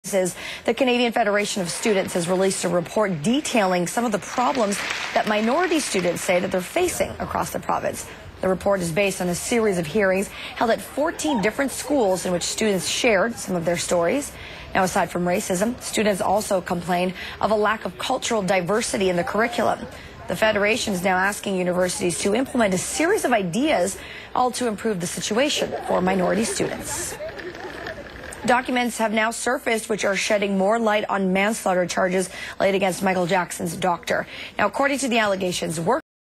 Task Force on Campus Racism report: CP24
Here is CP24's coverage of the release of the Final Report of the Task Force on Campus Racism. The Report was released to coincide with the International Day for the Elimination of Racial Discrimination